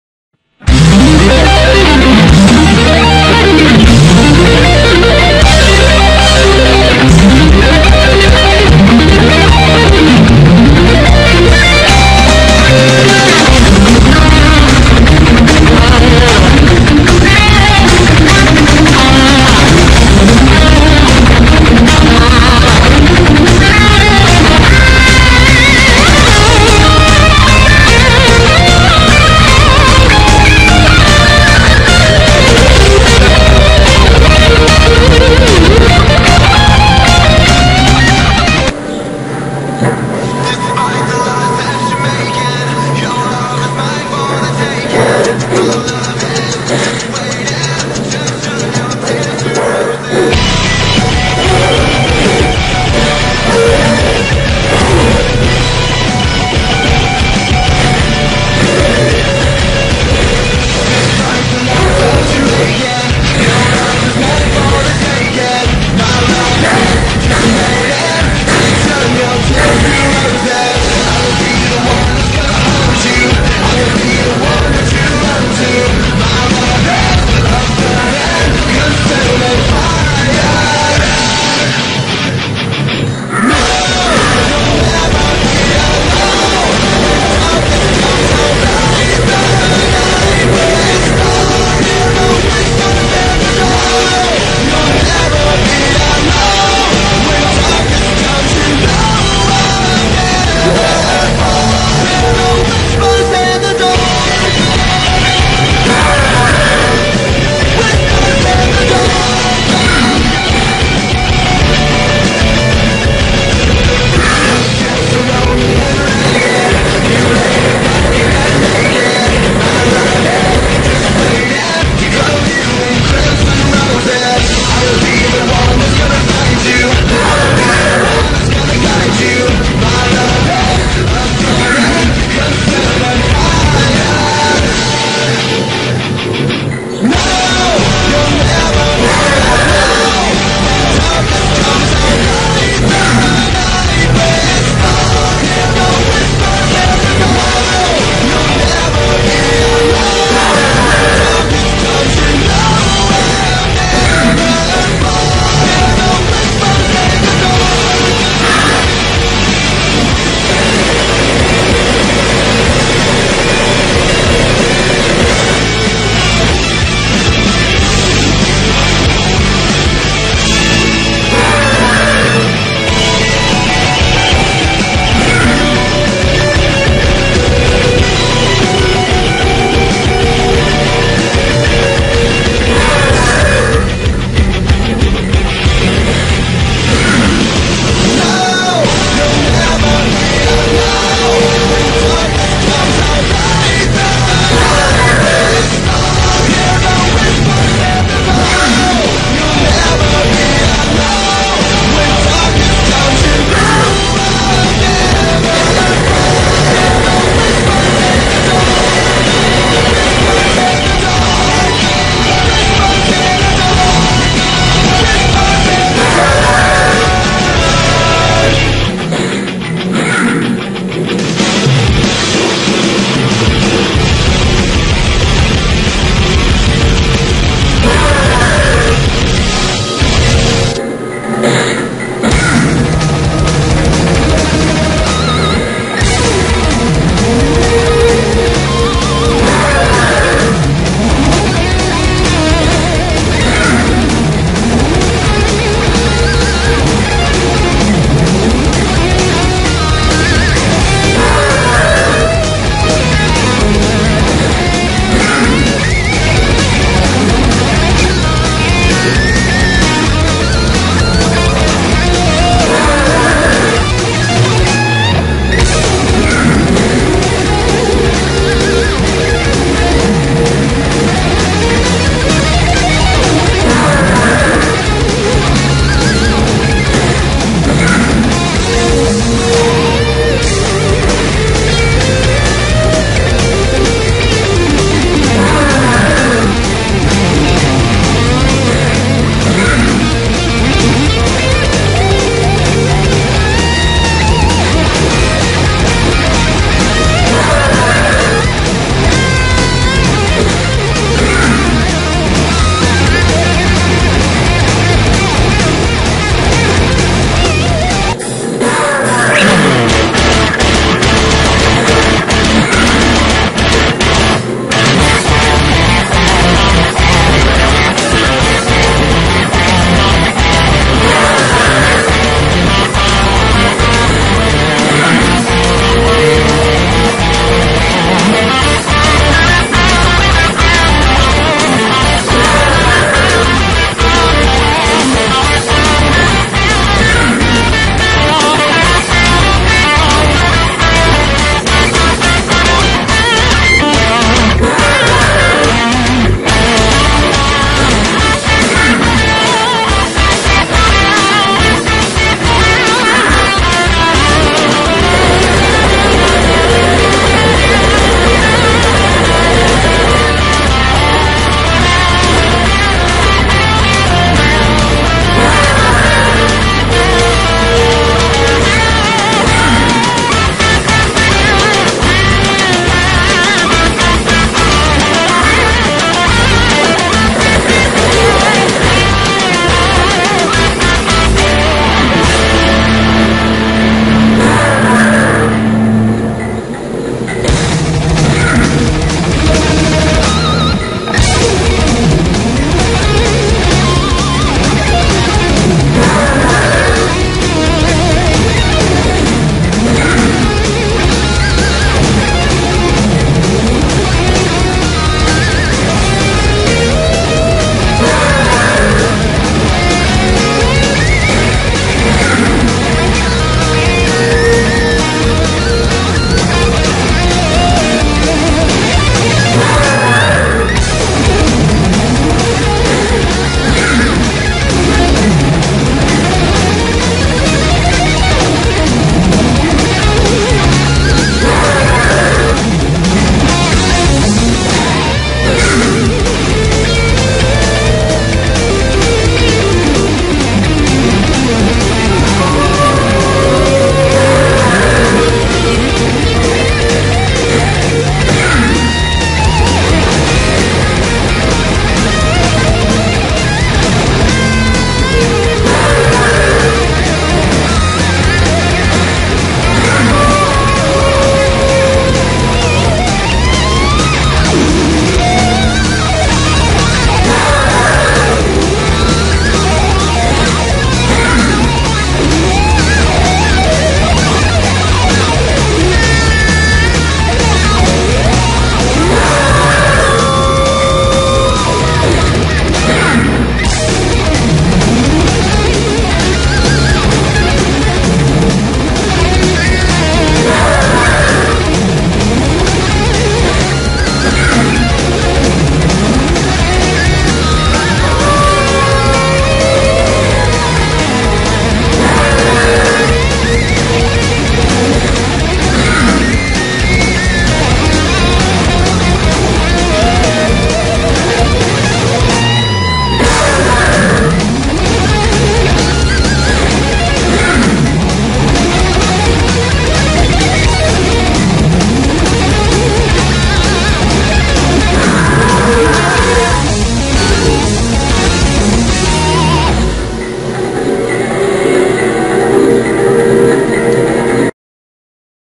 JJ morre na faca
Irei mostrar o vídeo completo de como matar o J.J só com a faca. Comentem e avaliem.